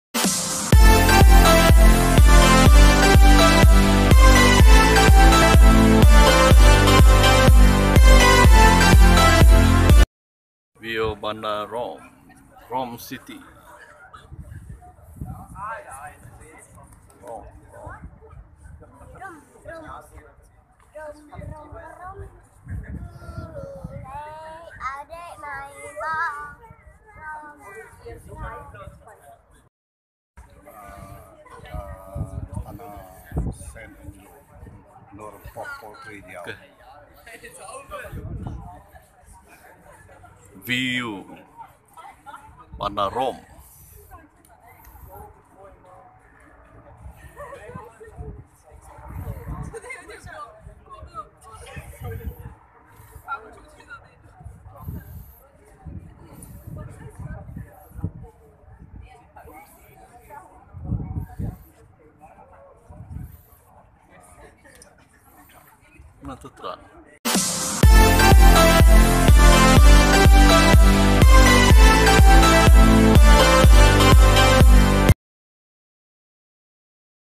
Rome City View
This video is a Phelleas family vacation in Italy and this time that visited Sant' Angelo's   Palace and went upstairs once to enjoy the beauty of Rome from the top of the building.